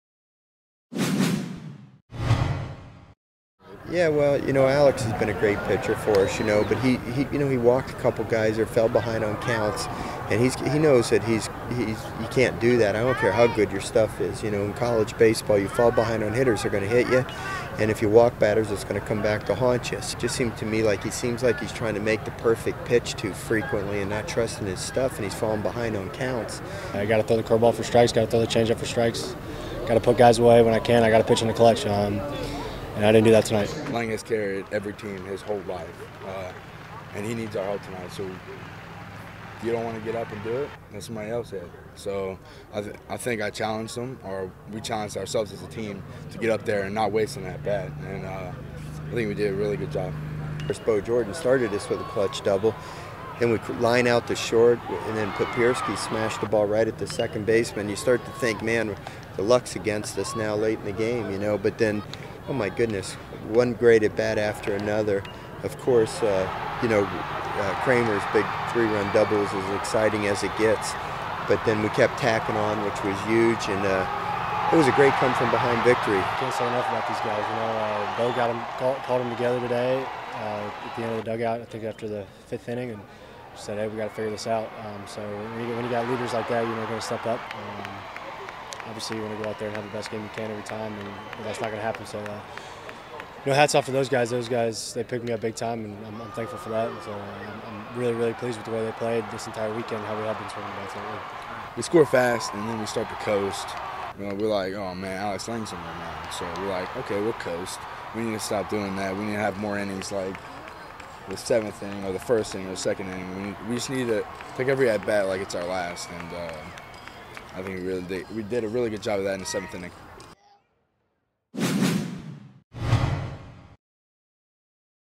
Baseball completes series sweep with 10-7 comeback victory
Saturday's series finale was far from what LSU is used to seeing from sophomore pitcher Alex Lange. He allowed 5 earned runs in 5.2 innings. When he left the game, he was on the hook for the loss.

But lucky for him, his teammates had a huge inning in the seventh to help him avoid his first career loss.